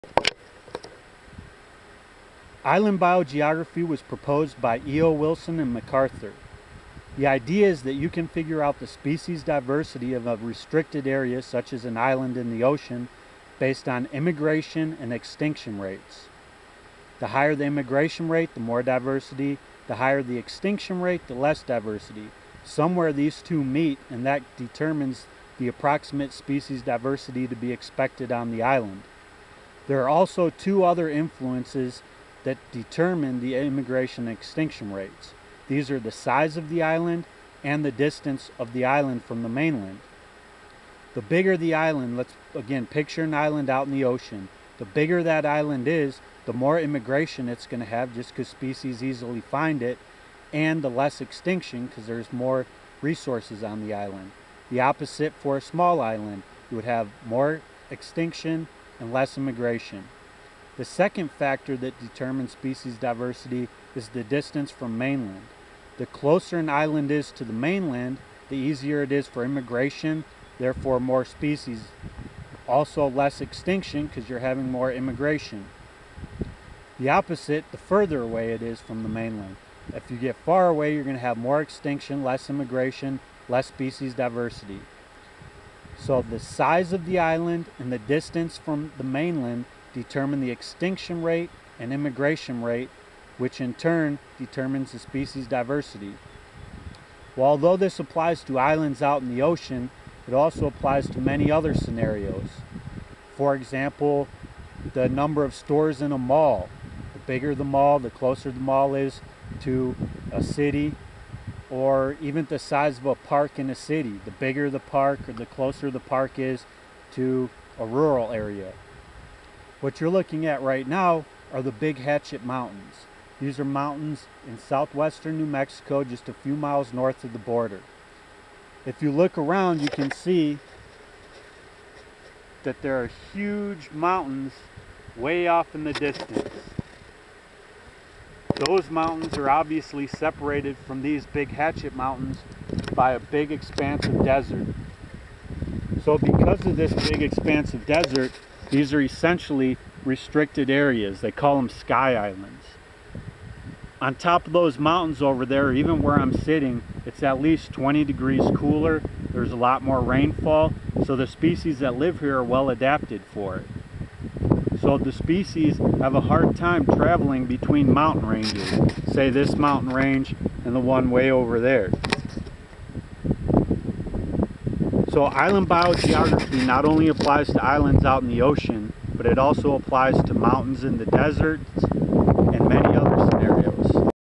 Island Biogeography
A short video describing the theory of Island Biogeography